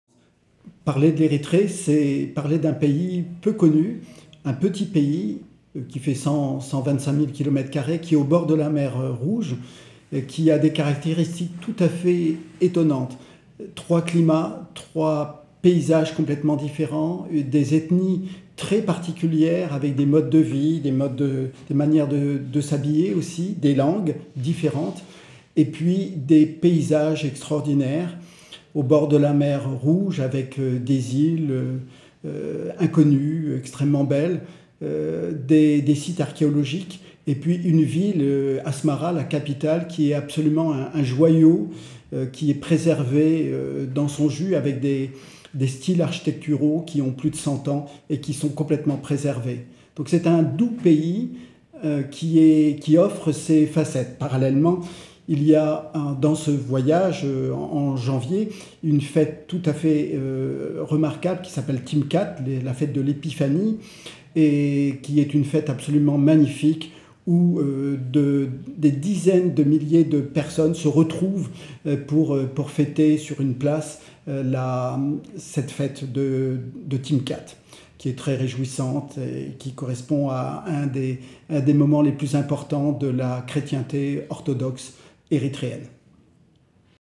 José-Marie Bel vous emmène en Erythrée pour les fêtes de Timkat
Ecrivain voyageur et conférencier qui compte parmi les plus éminents spécialistes de la Péninsule Arabique et de la Corne de l’Afrique, José-Marie Bel conduira le prochain voyage en Erythrée et dévoilera ainsi les secrets de la « perle cachée » de cette région.